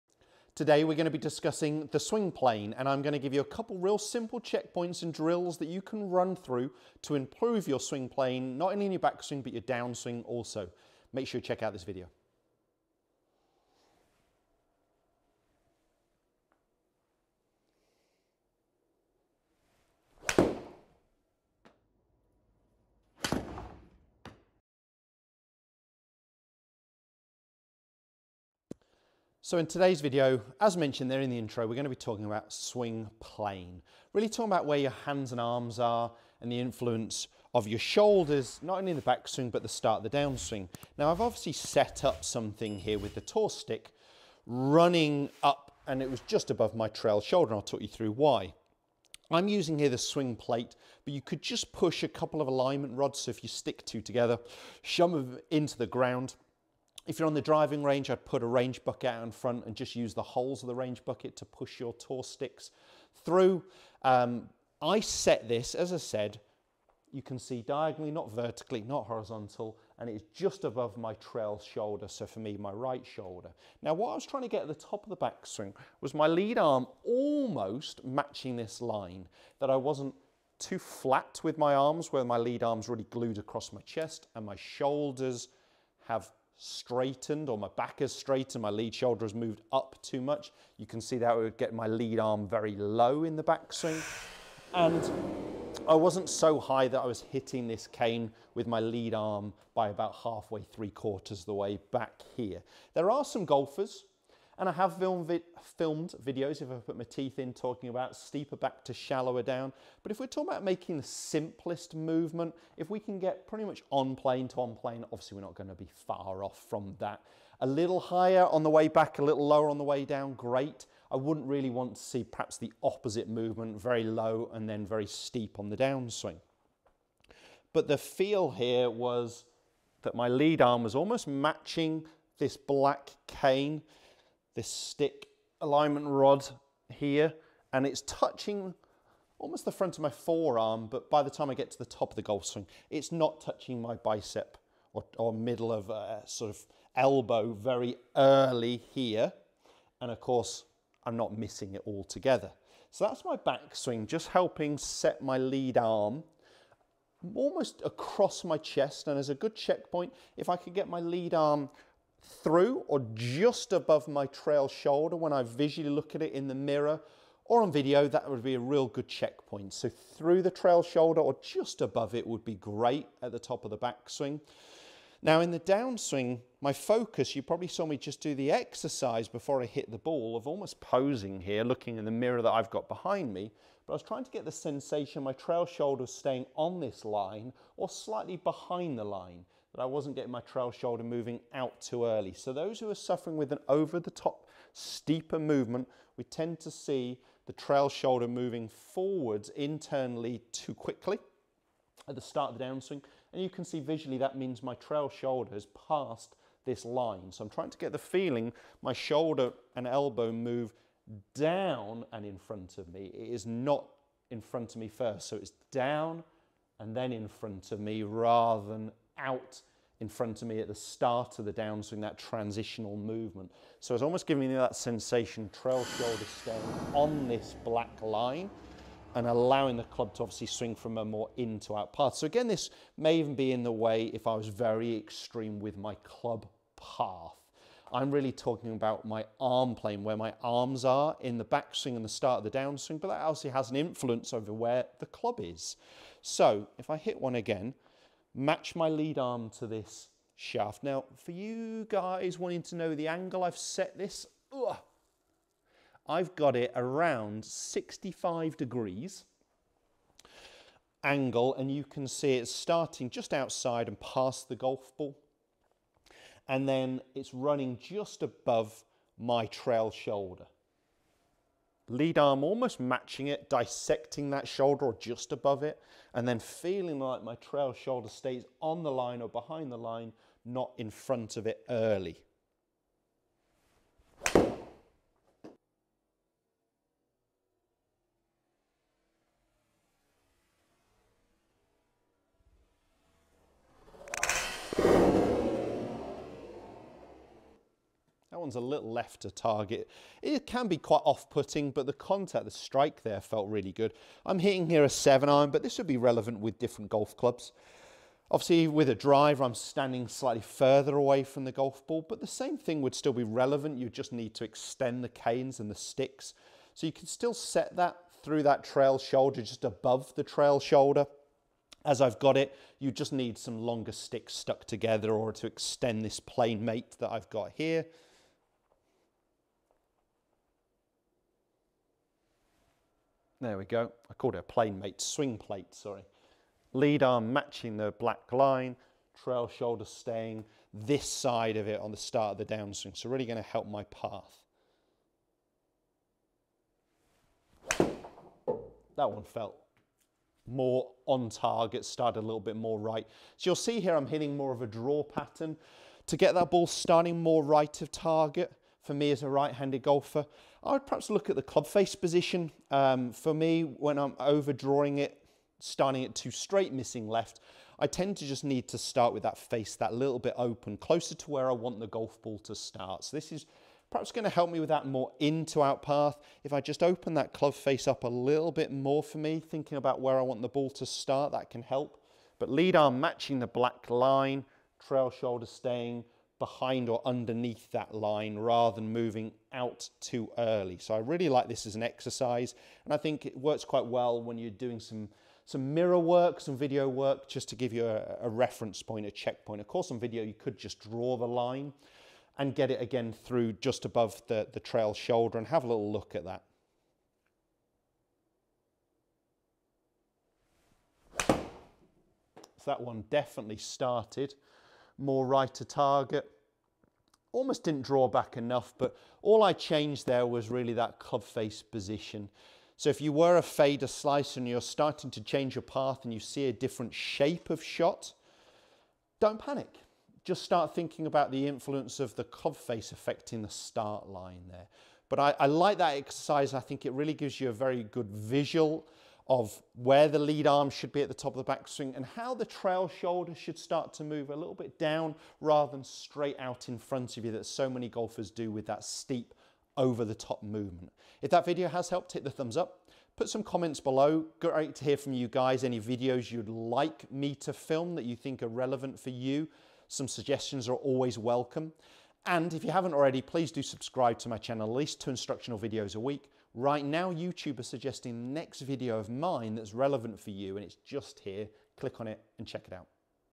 How to FIX Your Swing Plane : Simple Solution
How to get "On plane" in your golf swing. Simple tips to improve your back and downswing plane for a more reliable golf ball flight and consistent strike pattern.

TruGolf Academy is based at two amazing venues. Indoor studio in Barton Under Needwood and specific short game area in Whittington. Staffordshire, UK.